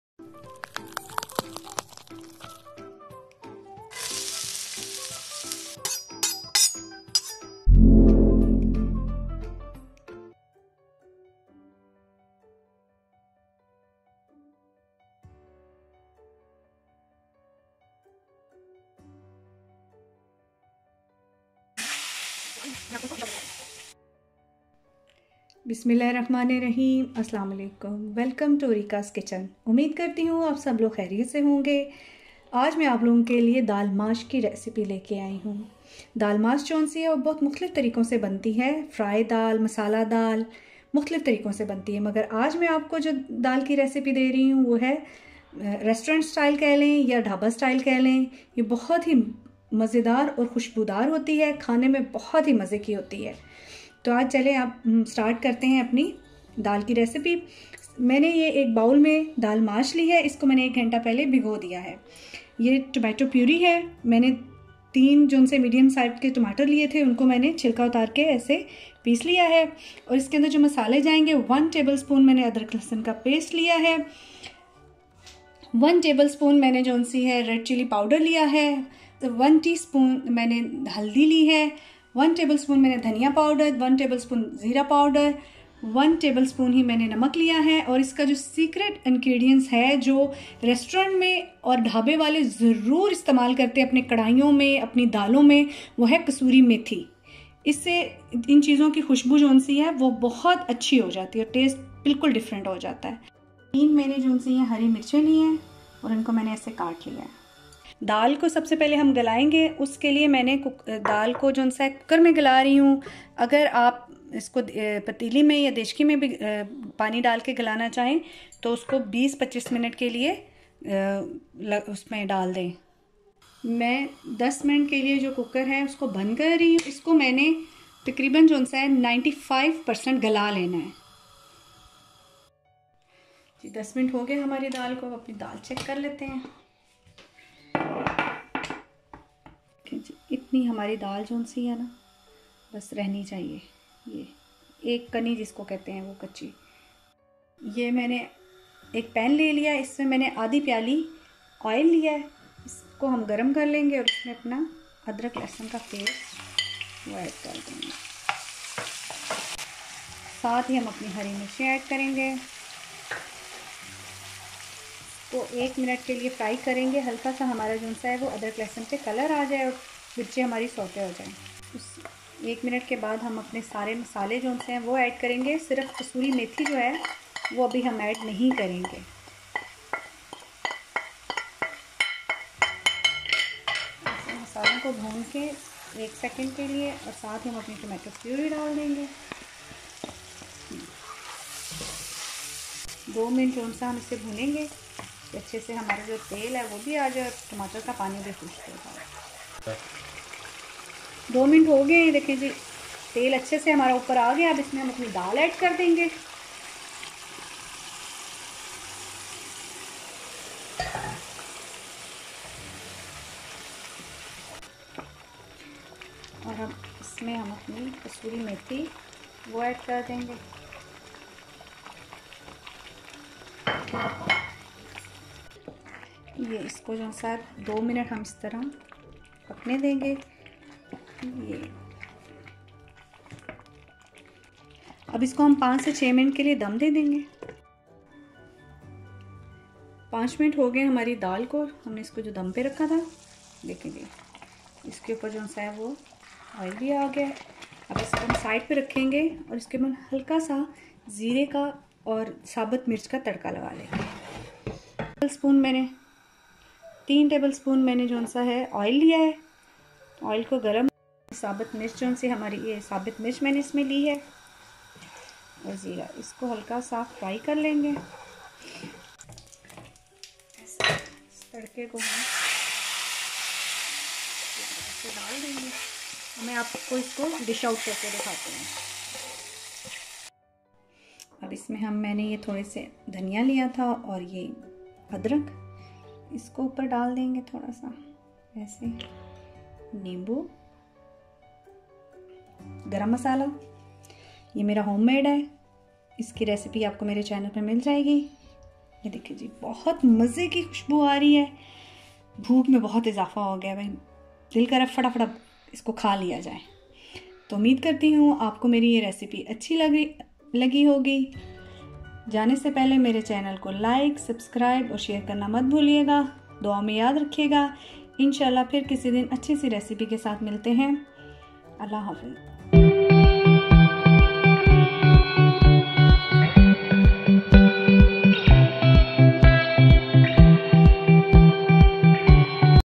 Daal Mash Dhaba Style || Daal Mash Resturant Style Recipe By Arika´s Kitchen
Daal Mash is liked by almost every person and made by several ways, but today recipe of Daal Mash is esp of Dhaba Style , easily available in Resturants too. It is very easy and you can take a tast at home of same Daal. :):.